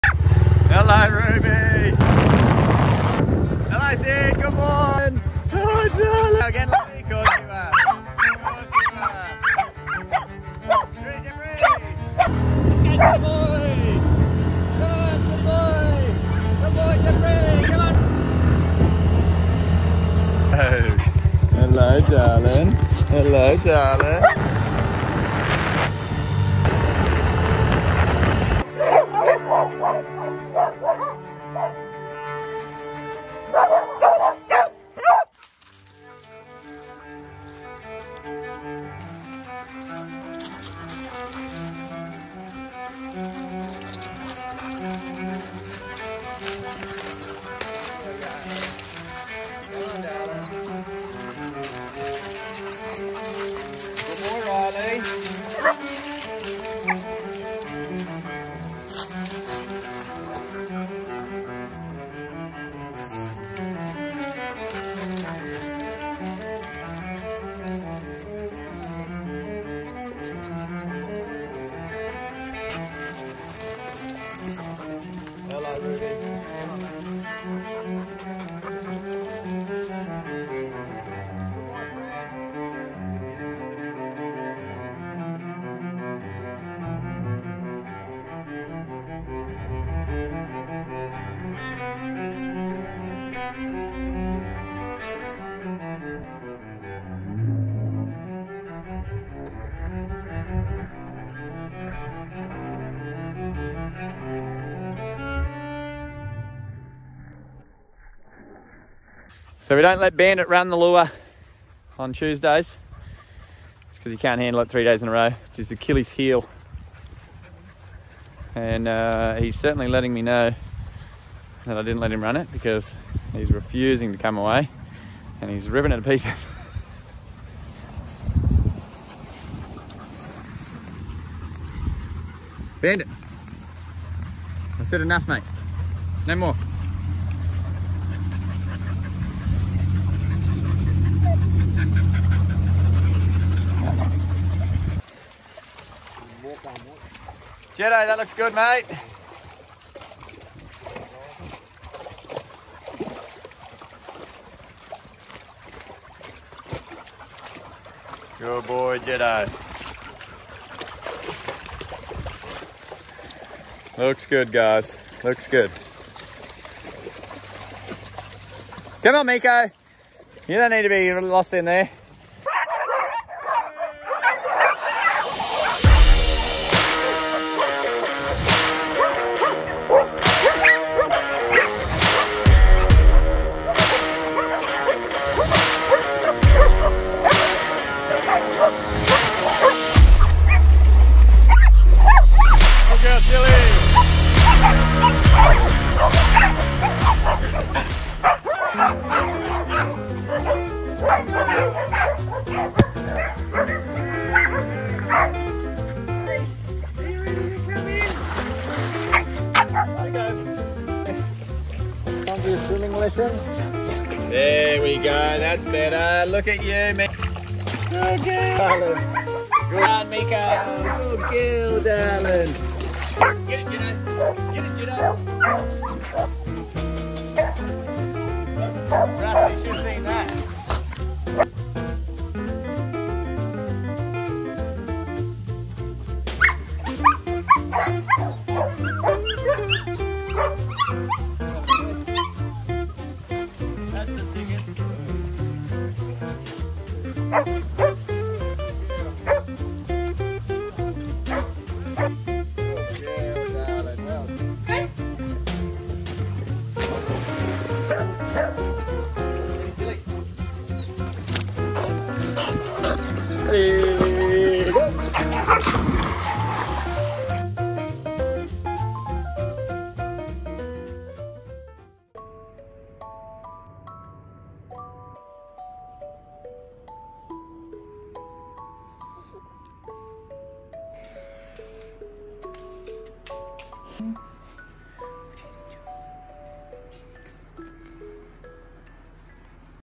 Sid the Airedale Terrier is fast against the motorbike in the morning
Sid the Airedale Terrier went hard this morning straight off the bus and into the sprints against Uncle Luke on the motorbike. So to was Louis the Border Collie, Ruby the Boxer and Taz the Wheaten Terrier.

On the lure course there were several dogs that nabbed to lure as well as a few that went through their legs!

In the heated pool, Meeko the Pug continued her swimming lessons and Jed the Border Collie ate his splashes.

After a big day at the farm, it was time to settle in for a cosy evening together.